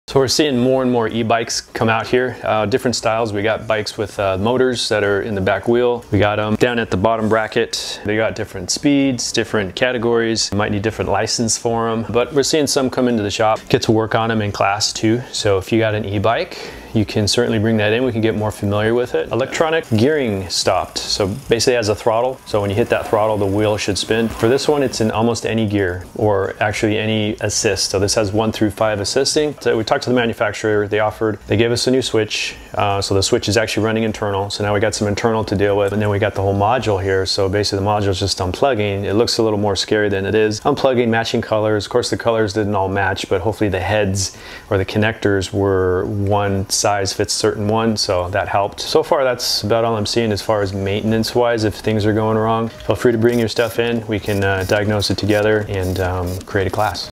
E-Bike (electric) Class and service at Bike Teacher in San Jose CA
E-Bike Class and Service at Bike Teacher in San Jose CA. Bring your electric bike in for service or learn about it in class and how to service and repair it. Any brand and any style e-bikes are welcome. I also will convert your ​bicycle to an e-bike with some of the kits that are offered.